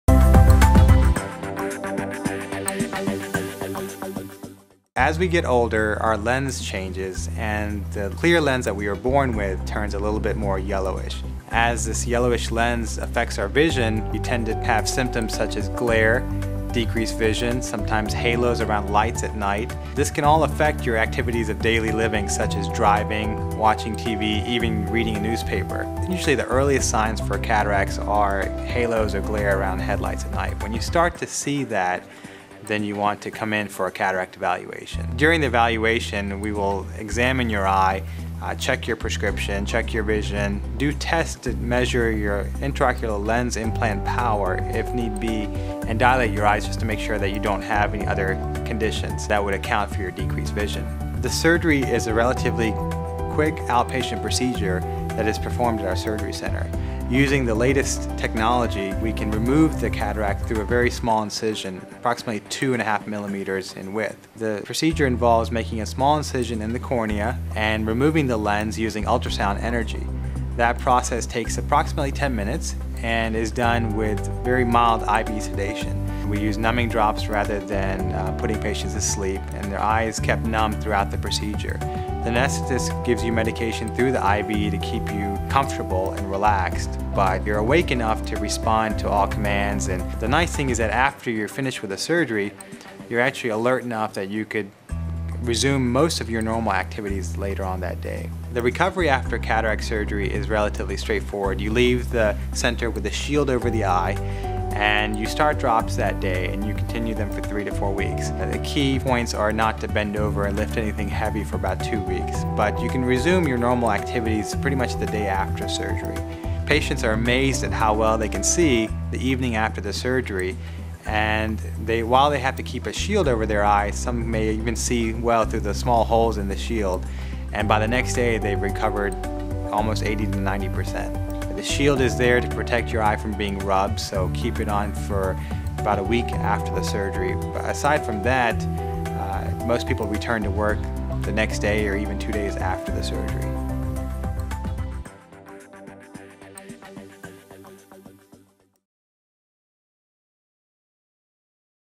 Definitive Productions: What Are Cataracts and Cataract Surgery?
Video produced for Milan Eye Center in Johns Creek, GA. Video features Milan Patel, M.D., explaining what causes cataracts, symptoms of cataracts, and what we can do to treat cataracts.

What are symptoms of cataracts?

As we get older our lens changes and the clear lens we were born with turns a little bit more yellowish. As this yellowish lens affects our vision you tend to have symptoms such as glare, decreased vision, sometimes haloes around lights at night. This can all affect your activities of daily living such as driving, watching television, even reading the newspaper. Usually the earliest signs for cataracts are haloes or glare around headlights at night. When you start to see that, then you want to come in for a cataract evaluation.
What is a cataract eye exam?

During the evaluation, we will examine your eye, check your prescription, check your vision, do tests to measure your intraocular lens implant power if need be. And dilate your eyes just to make sure that you don't have any other conditions that would account for your decreased vision.
Surgery for Cataracts
The surgery is a relatively quick outpatient procedure that is performed at our surgery center.  Using the latest technology, we can remove the cataract through a very small incision, approximately 2.5mm in width. The procedure involves making a small incision in the cornea and removing the lens using ultrasound energy. That process takes approximately ten minutes and is done with very mild IV sedation. We use numbing drops rather than putting patients to sleep and the eye is kept numb throughout the procedure. The anesthetist gives you medication through the IV to keep you comfortable and relaxed but you're awake enough to respond to all commands.
Recovery Time from Cataract Surgery
The nice thing is that after you're finished with the surgery, you're actually alert enough that you could resume most of your normal activities later on that day. The recovery after cataract surgery is relatively straight forward. You leave the center with a shield over the eye and you start eye drops that day that you continue for them three to four weeks. The key points are not bend over and lift anything heavy for about two week. But you can resume your normal activities pretty much the day after surgery. Patients are amazed at how well they can see the evening after the surgery. While they have to keep a shield over their eye, some may even be able to see well through the small holes in the shield. And by the next day they've recovered by almost 80-90%. The shield is to protect your eye from being rubbed. So, keep it on for a week after the surgery. Aside from that most people return to work the next day or two days after surgery.